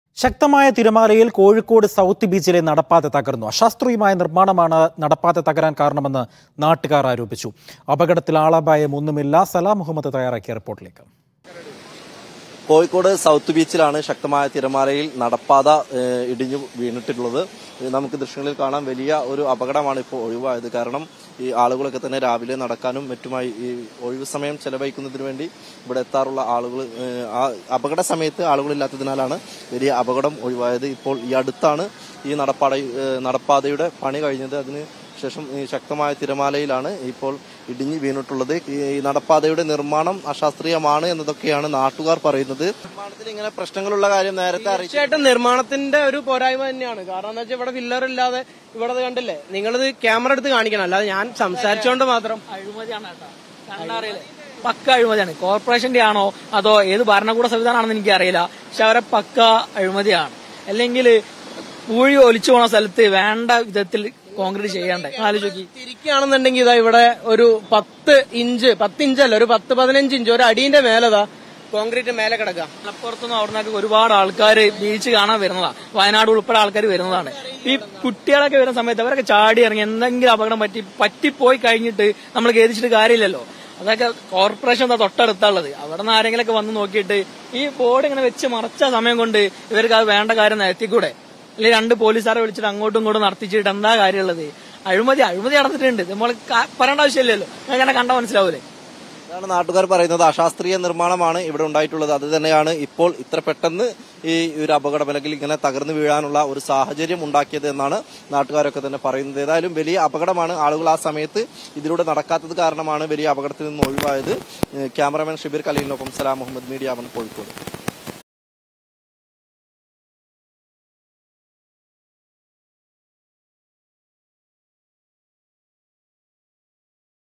കോഴിക്കോട് സൗത്ത് ബീച്ചിലെ നടപ്പാത തകർന്നു; അപകടത്തിൽ ആളപായമില്ല
ശക്തമായ തിരമാലയിൽ കോഴിക്കോട് സൗത്ത് ബീച്ചിലെ നടപ്പാത തകർന്നു. അശാസ്ത്രീയ നിർമാണമാണ് നടപാത തകരാൻ കാരണമെന്ന് നാട്ടുകാർ ആരോപിച്ചു. അപകടത്തിൽ ആളപായം ഒന്നുമില്ല.

Genre: News
Language: Malayalam